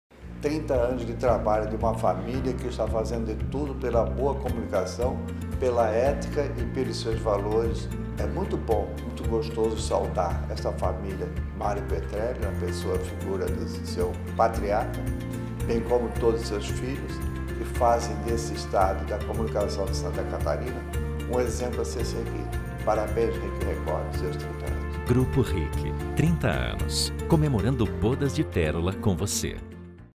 Grupo RIC 30 anos: Depoimento Presidente lojas Koerich
Em 2017 o Grupo RIC comemora 30 anos de trabalhos desenvolvidos. Como comunicação não se faz sozinho, a empresa buscou ouvir a opinião de importantes entidades que costumam estar no seu conteúdo editorial.  Confira o vídeo de Antonio Koerich.